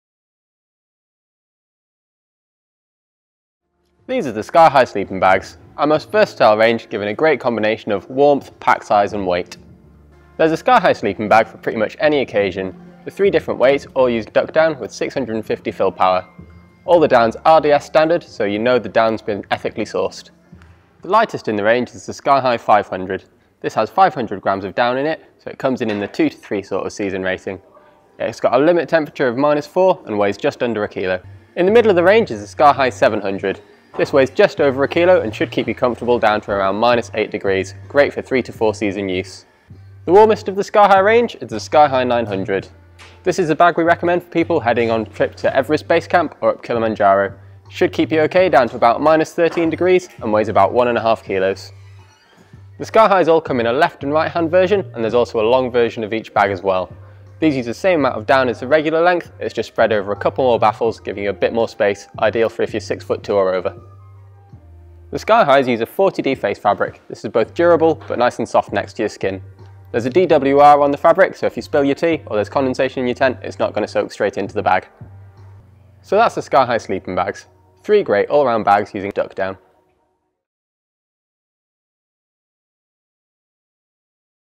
An Insight into the Alpkit SkyeHigh Sleeping Bag Range
SkyeHigh is our most versatile bag; light enough to carry up a mountain, compressible enough to stuff in a kayak and warm. It achieves the balance by using durable technical fabrics with a light DWR coating. Being made from 40D fabric, the SkyeHigh has the most attractive price tag maxing the warmth for your wallet.

Please check out the current product pages for current colourways and specs.